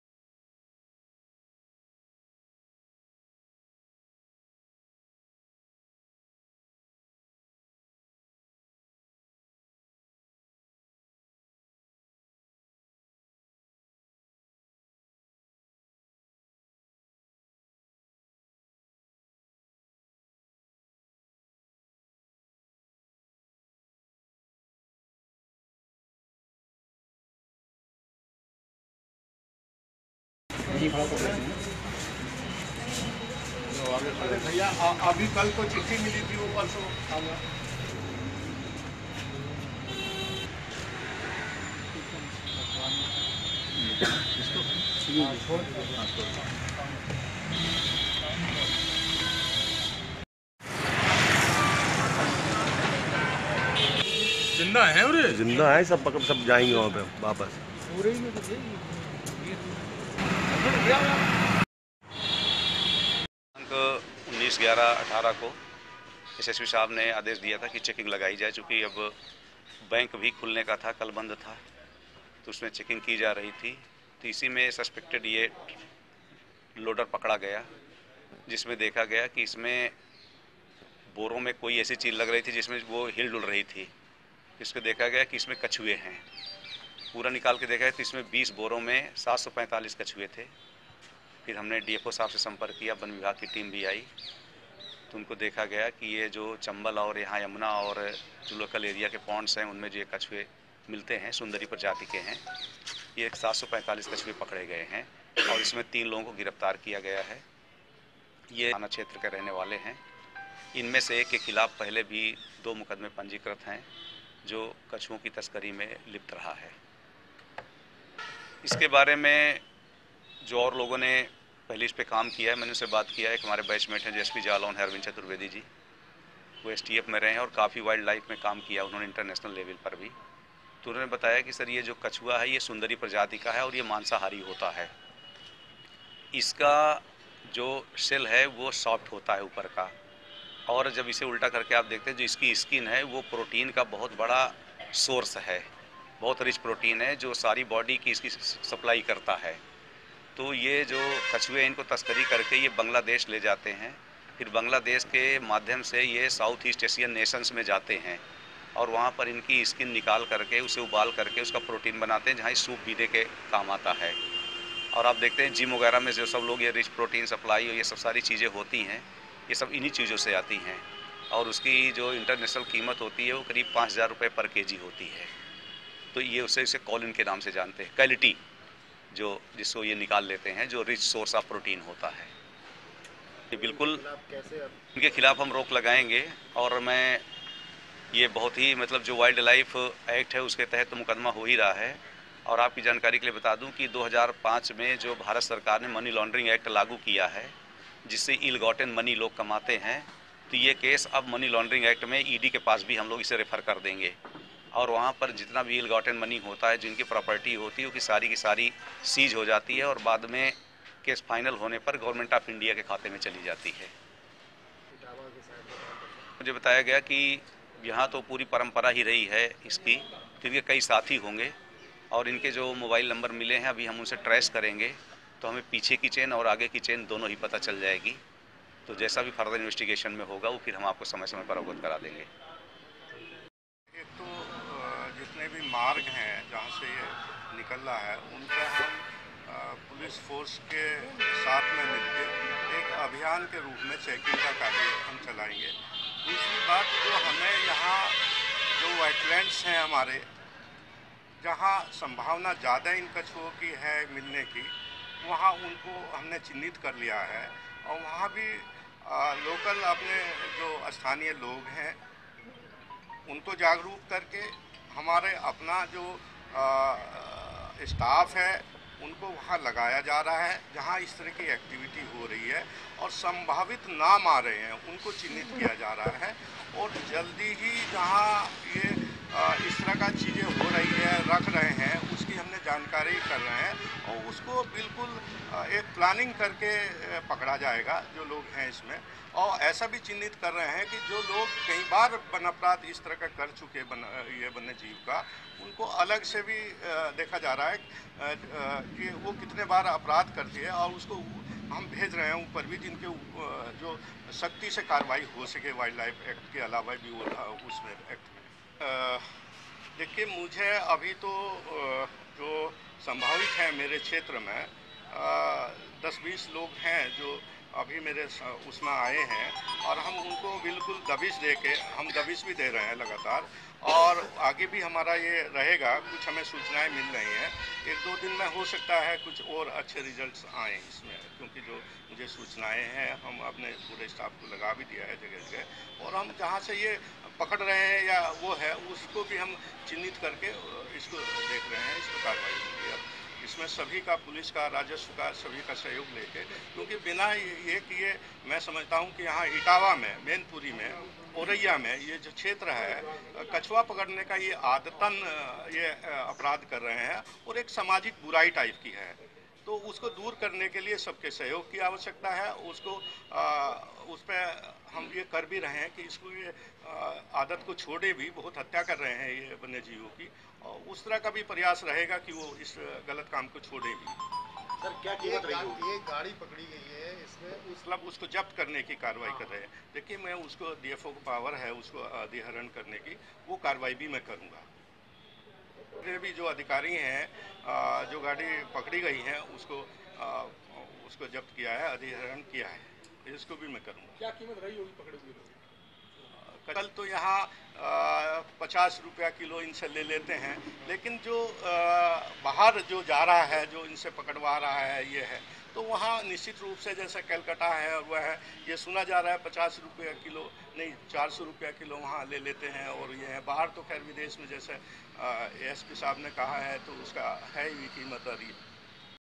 कछुओं की तस्करी करने वाले तीन आरोपी गिरफ्तार, 745 कछुए बरामद
इटावा: थाना सैफई पुलिस सोमवार सुबह दुमिला गांव के पास वाहन चेकिंग कर रही थी. पुलिस ने सामने से आते एक लोडर को रोककर चेक किया तो उसमें बोरे में बंद अलग-अलग प्रजाति के 745 कछुए मिले. पुलिस ने कछुओं की तस्करी करने वाले चार तस्करों को हिरासत में ले लिया है.